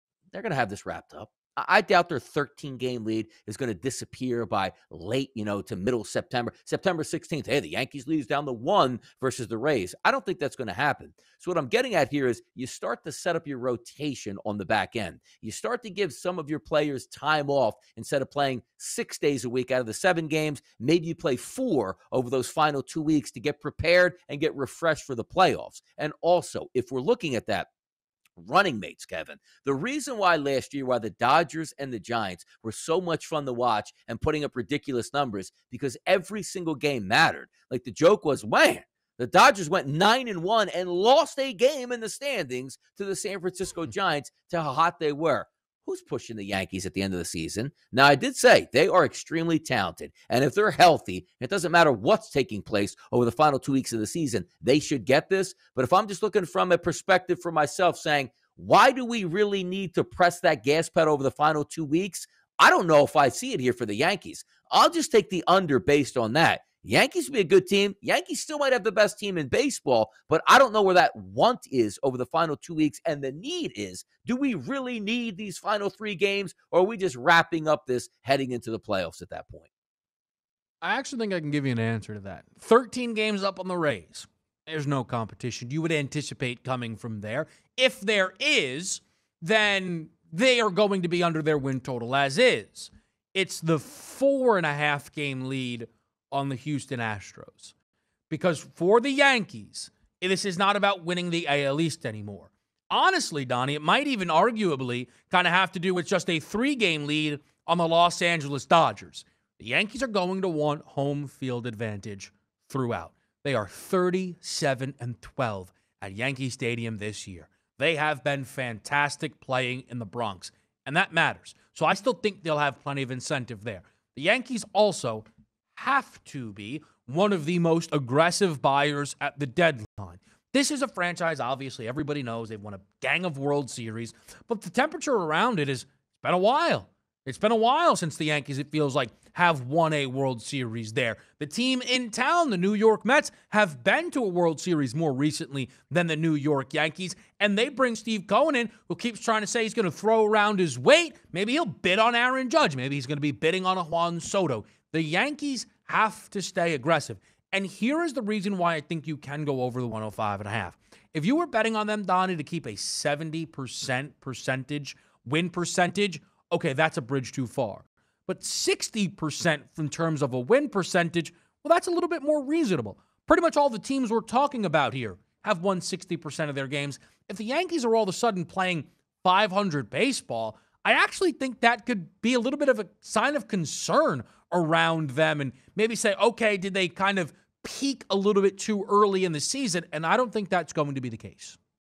Will The Yankees Go Over Their Current Win Total Of 105.5?
Kevin and Donnie break down the current win total of 105.5 for the New York Yankees on the FanDuel Sportsbook.